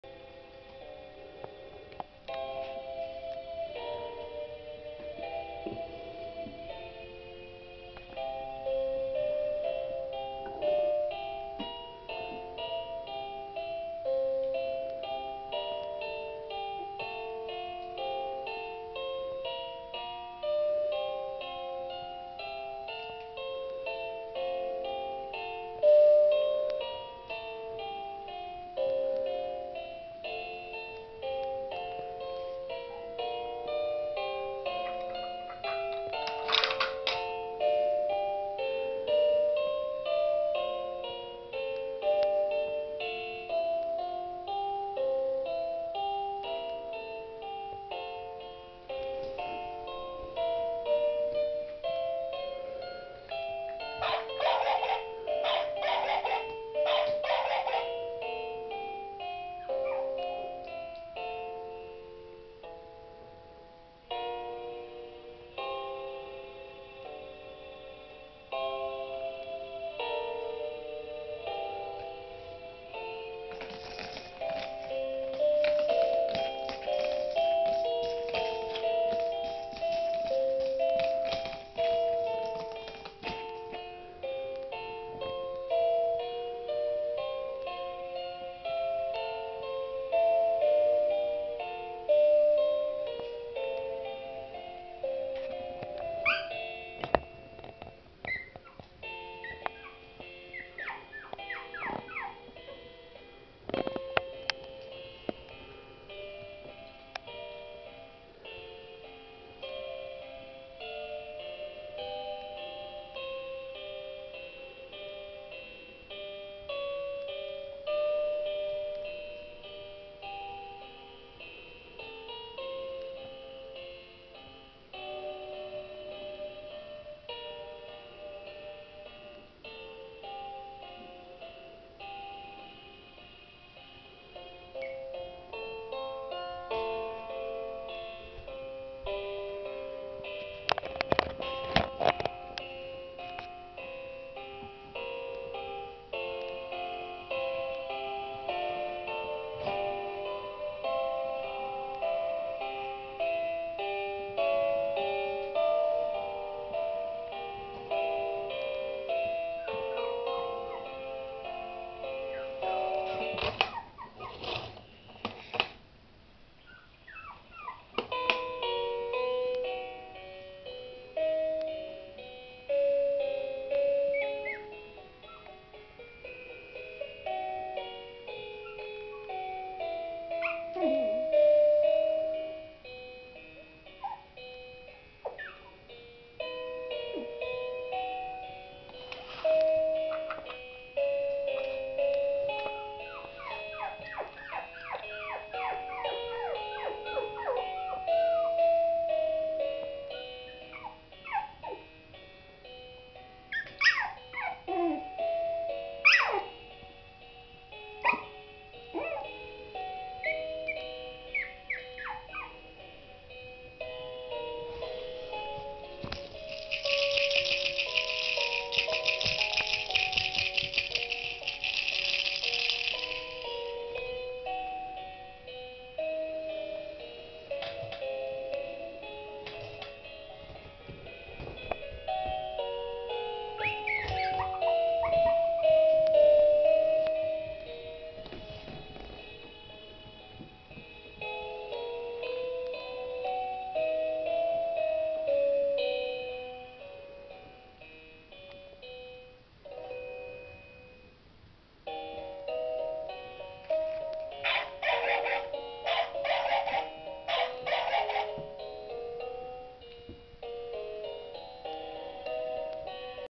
Lucy is a Maltipoo
This is Lucy she is a precious maltipoo puppy that is just about 6 weeks old.